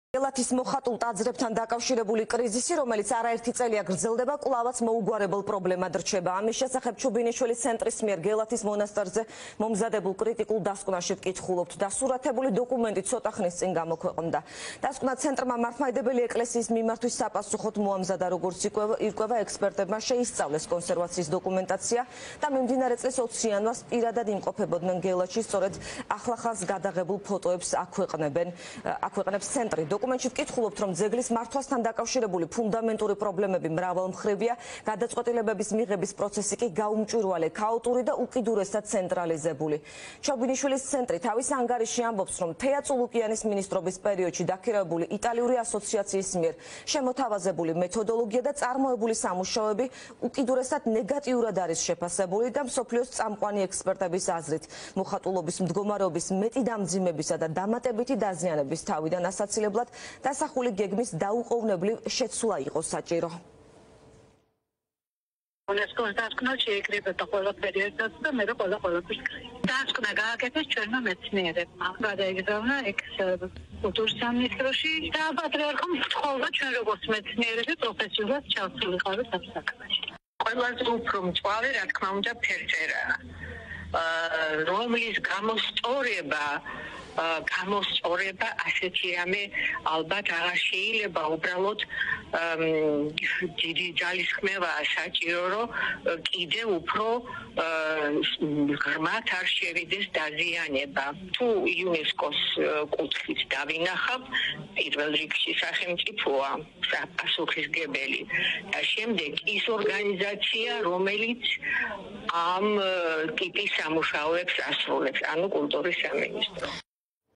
მორიგი კრიტიკული დასკვნა გელათზე - რა წერია დოკუმენტში?
▪️ „გელათის მოხატულ ტაძრებთან დაკავშირებული კრიზისი კვლავაც მოუგვარებელ პრობლემად რჩება!“ — ჩუბინიშვილის ცენტრის მიერ გელათის მონასტერზე მომზადებული კრიტიკული დასკვნა, რა წერია დოკუმენტში?